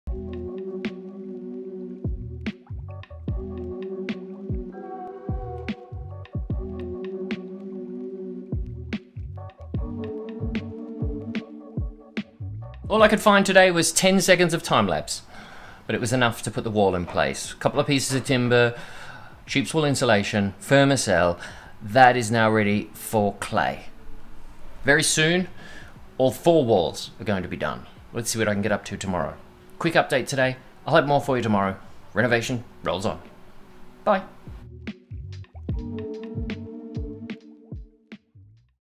Renovation: last wall ready for clay plastering in the WC.
Only 10 seconds of time lapse available for renovation today. But it was enough to get the small section of wall above the WC door ready for clay.